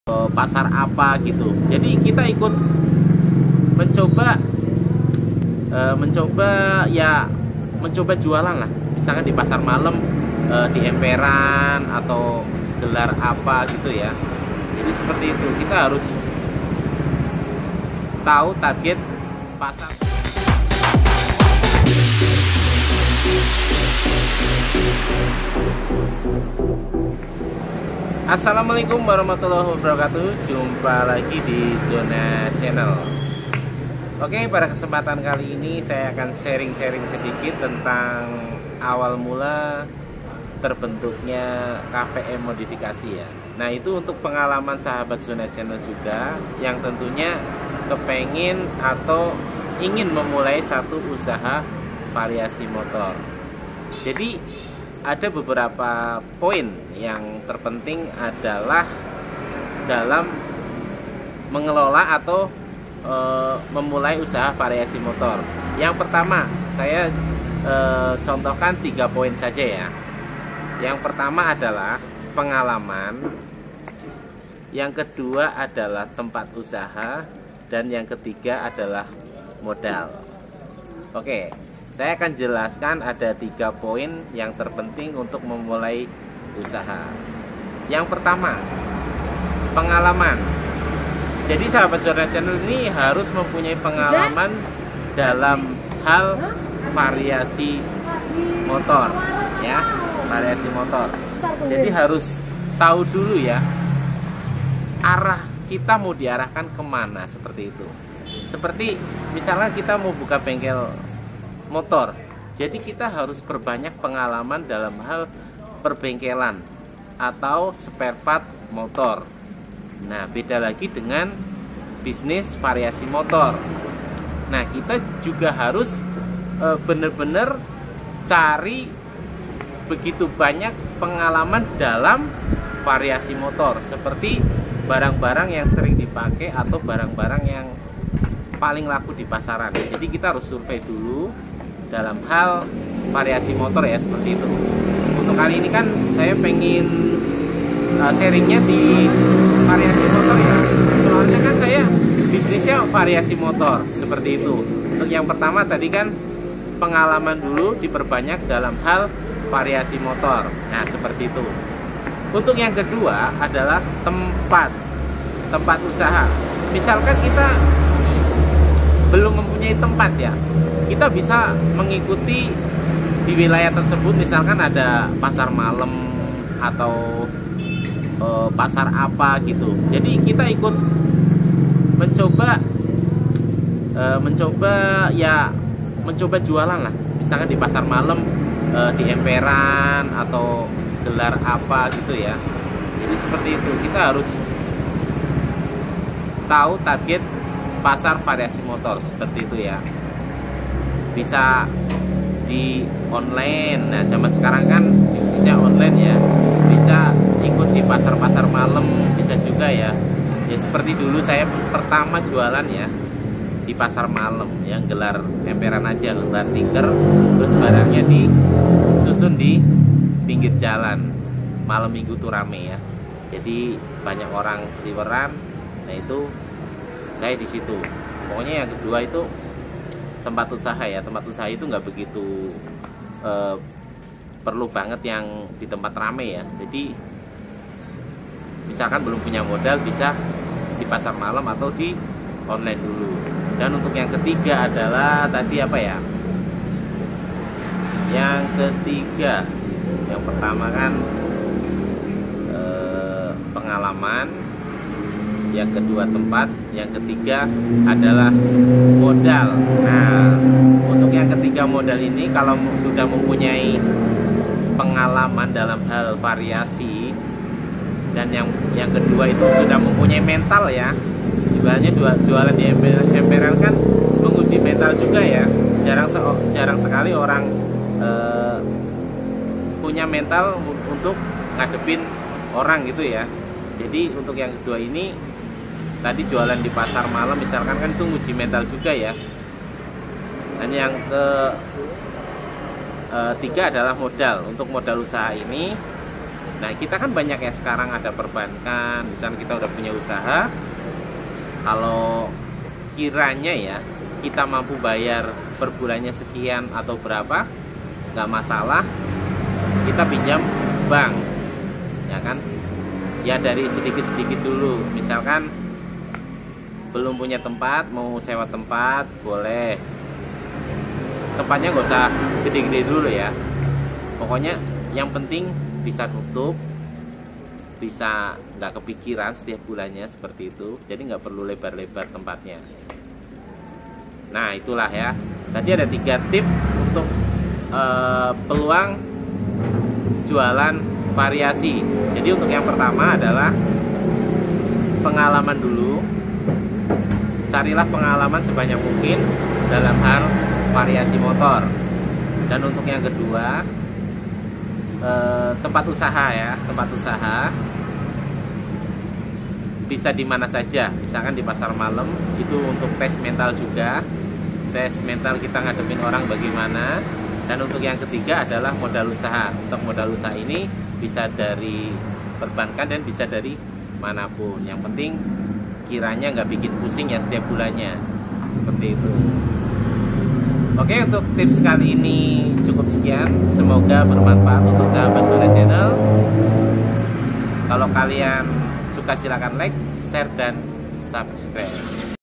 3 TIPS YANG HARUS DIPERHATIKAN DALAM MEMULAI USAHA VARIASI MOTOR
kali ini saya akan sharing tentang 3 cara yang harus diperhatikan dalam memulai usaha variasi dan modifikasi motor.untuk pemula ya wajib menonton